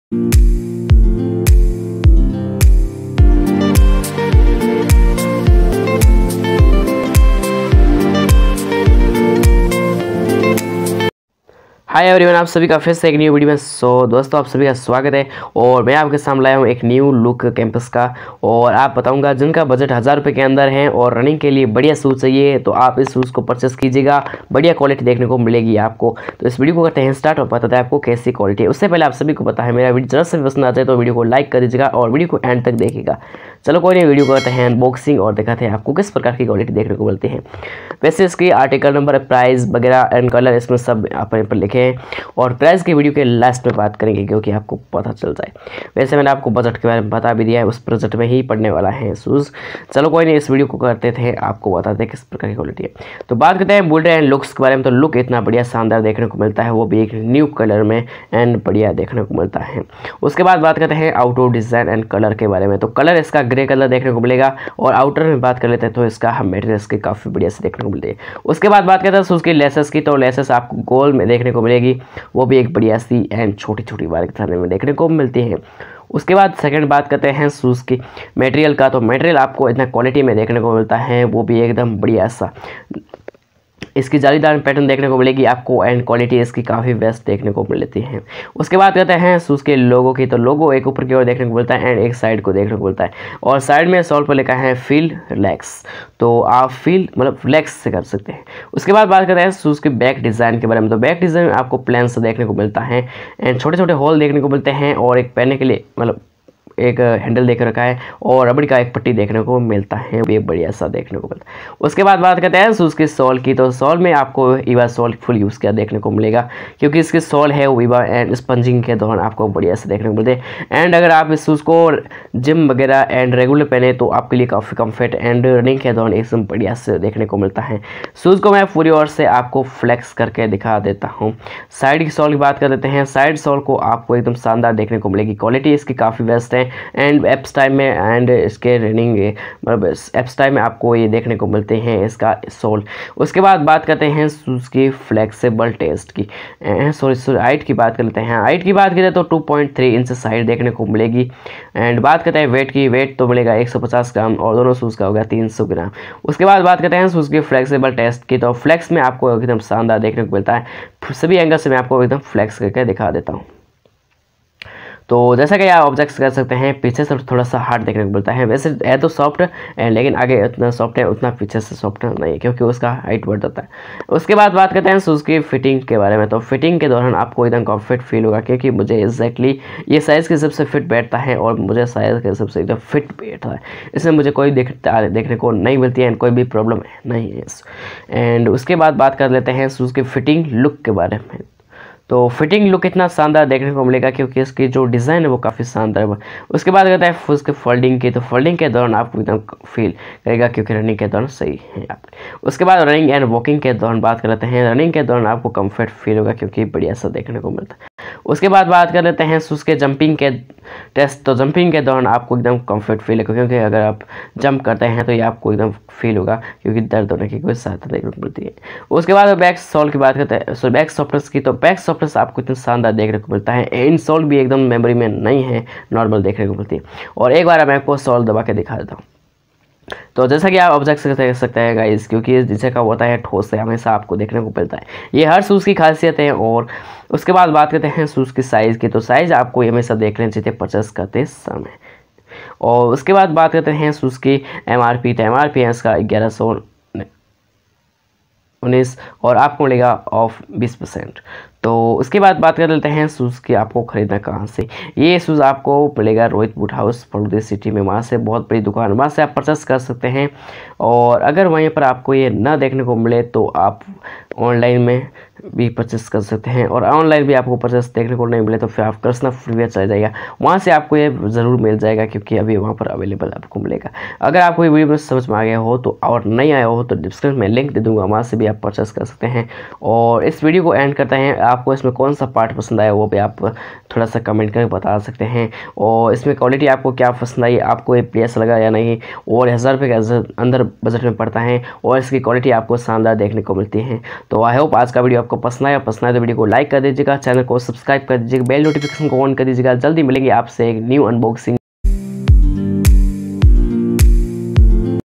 CAMPUS KA KAM PRICE ME SHOES RUNNING AND WORKOUT KE LIYE BEST SHOES FULL DETAILS LIGHT WEIGHT..2022
COLOUR GRY/RED
ARTICLE NAME CLUSTER PRO
QUALITY..
SHOES KI QUALITY BAHUT BADIYA HAI AND RUNNING AND WORKOUT KE LIYE BAHUT SANDAR SHOES HAI EK BAR JARUR KHARIDE OR 1000 RU KE ANDER BAJAT WALO KE LIYE BADIYA SHOES HAI.
MY SHOP ADDRESS =ROHIT BOOT HOUSE [PHALODI]
🙏Jay hind
🙏Jay Bharat
🙏Jay kisan